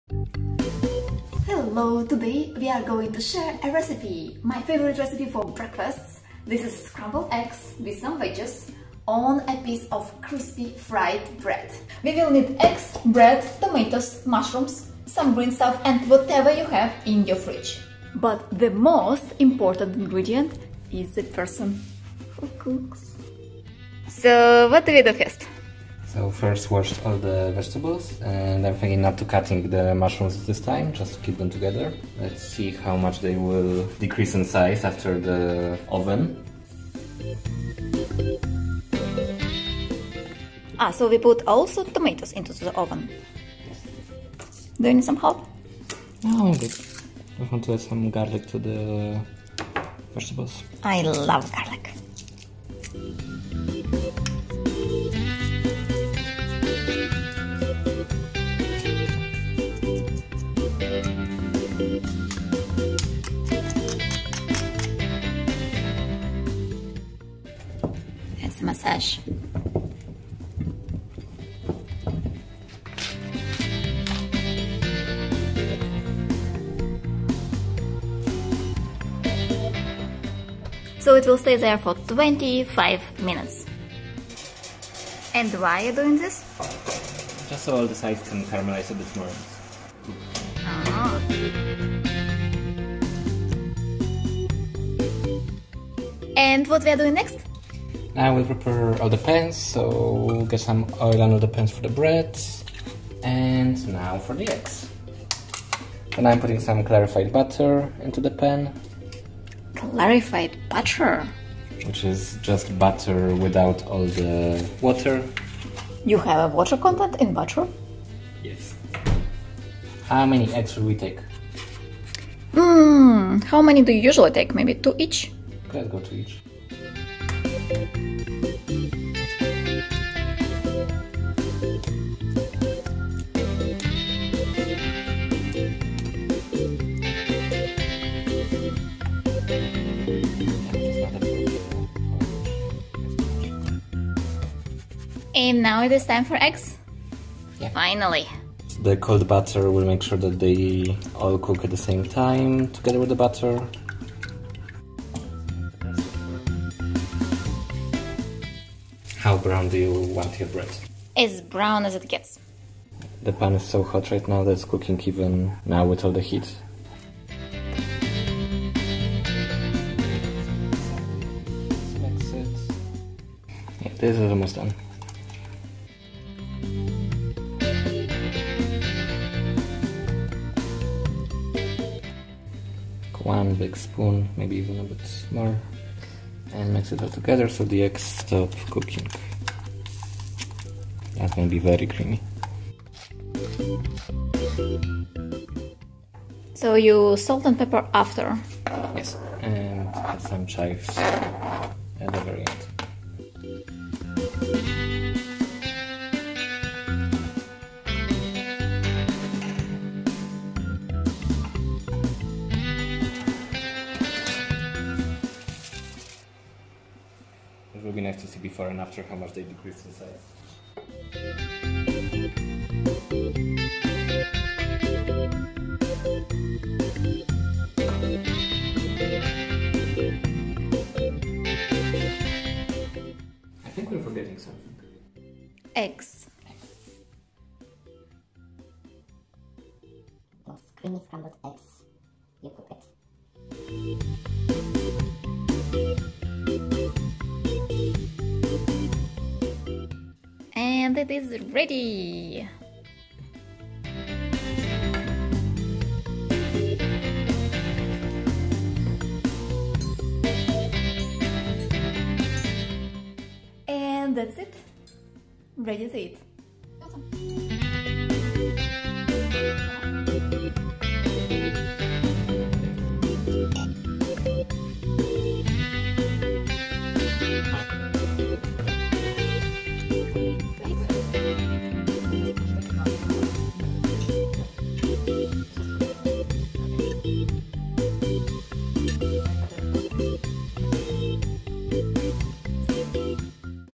Scrambled eggs breakfast
Simple breakfast - scrambled eggs on a crispy fried bread.